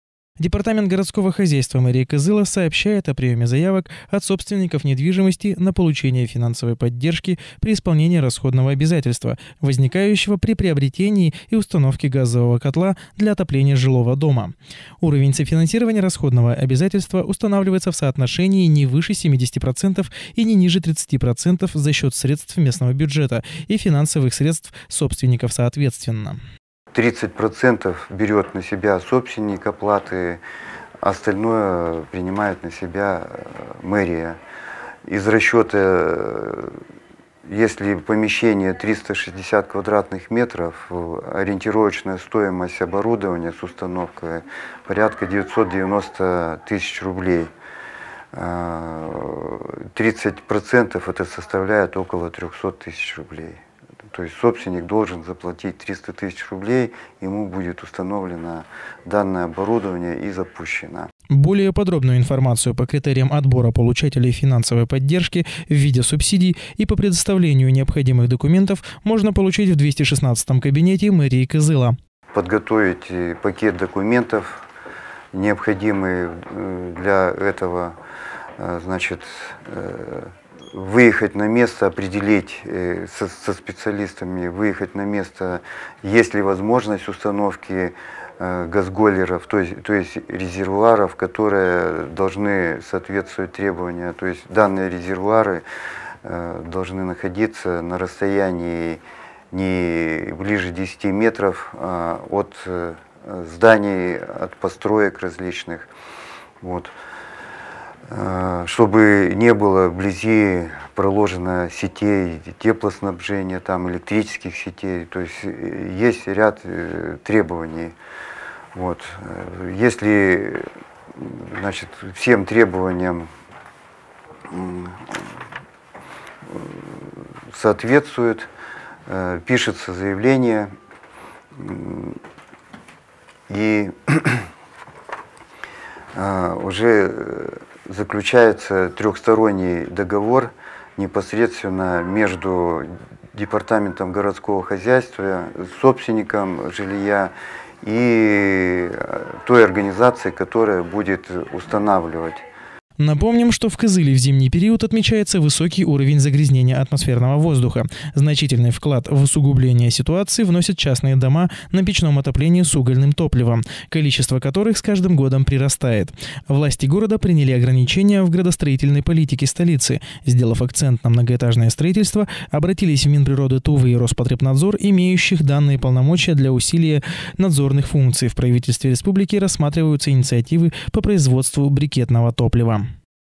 Газовое отопление вместо печного
Департамент городского хозяйства мэрии Кызыла сообщает о приеме заявок от собственников недвижимости на получение финансовой поддержки при исполнении расходного обязательства, возникающего при приобретении и установке газового котла для отопления жилого дома.